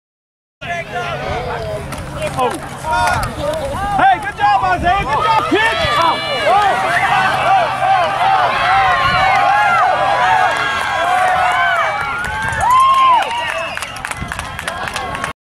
Isaiah Guerrero 49 yd Run 9/19/15
Isaiah Guerrero turns a high snap into a 49 yard scramble.